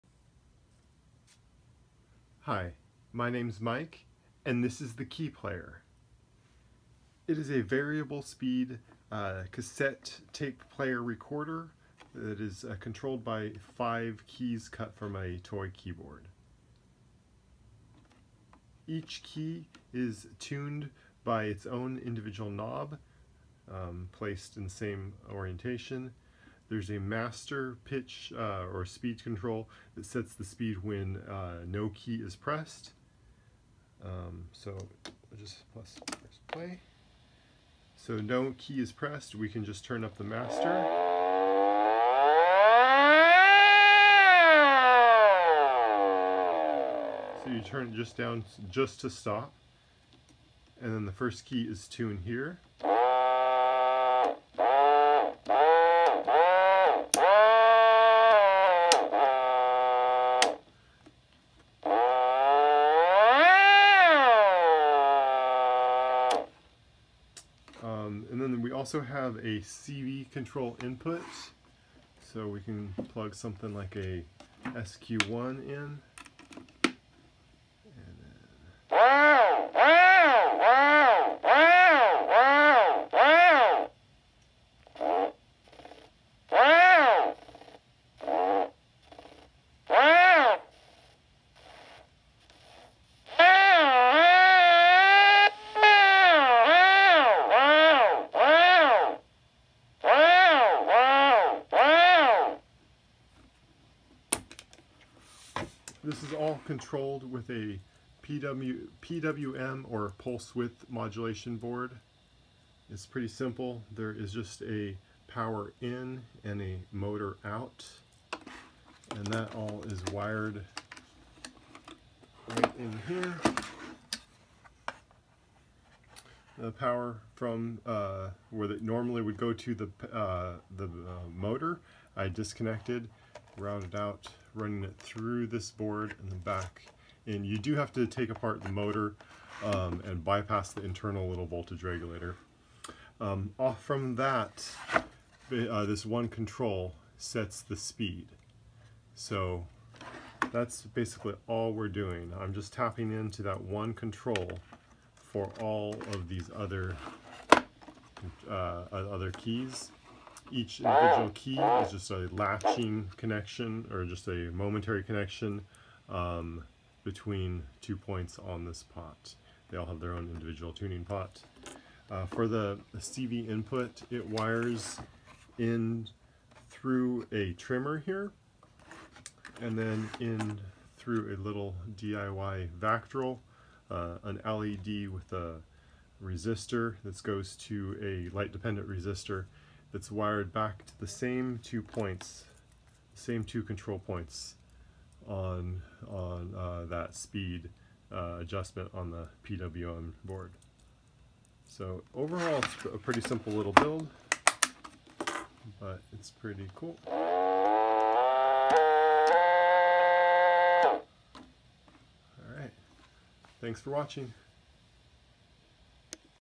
KeyPlayer - a circuit bent tape player instrument
Quick overview of my KeyPlayer device that I built. It is a circuit bent tape player controlled by a PWM board and five keys cut from a toy keyboard. Each key is freely tuned with it’s own control knob. Master control knob sets speed when no key is pressed. CV input jack sends signal through DIY vactrol externally set speed.